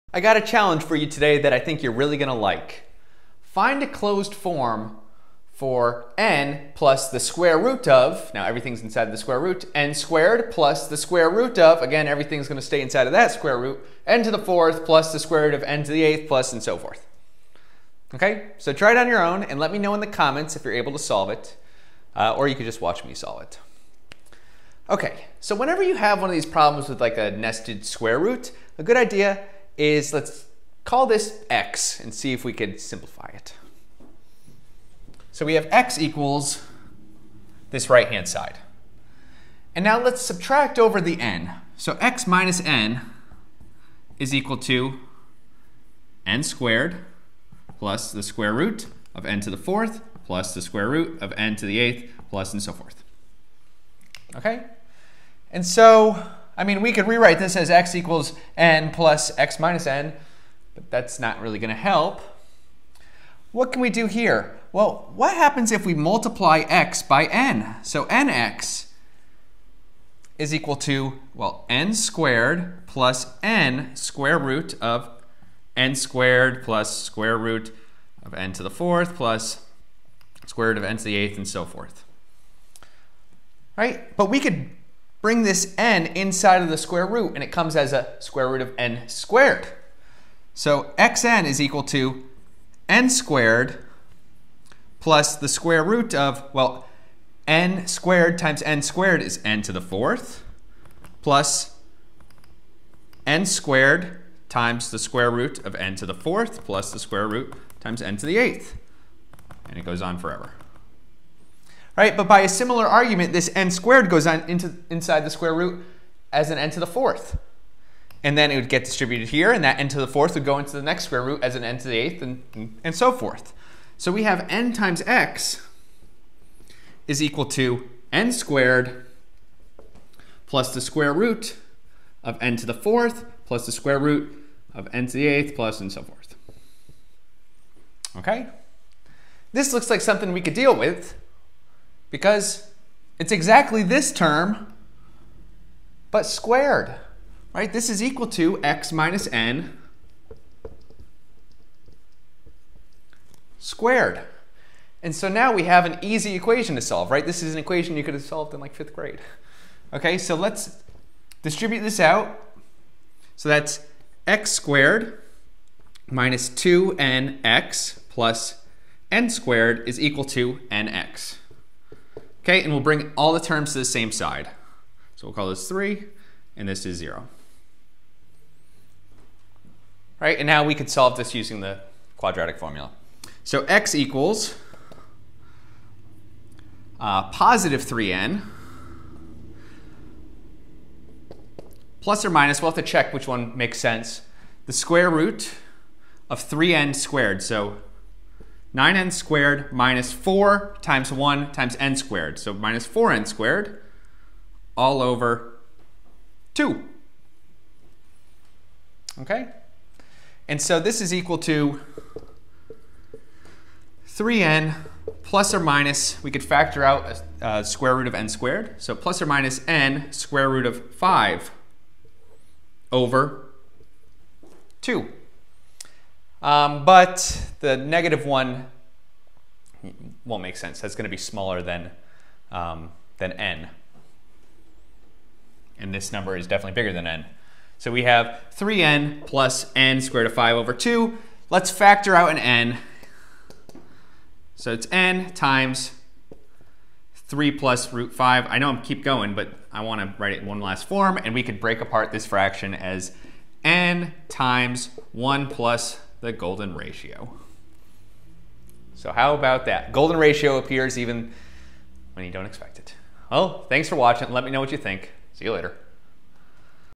🤔 I Wonder if I Could Have Solved This in Middle School 🤔
We find a closed form for n+ sqrt(n^2+sqrt(n^4+sqrt(n^8+...))).
Once you see how it's done, this question isn't very hard!

COMING SOON!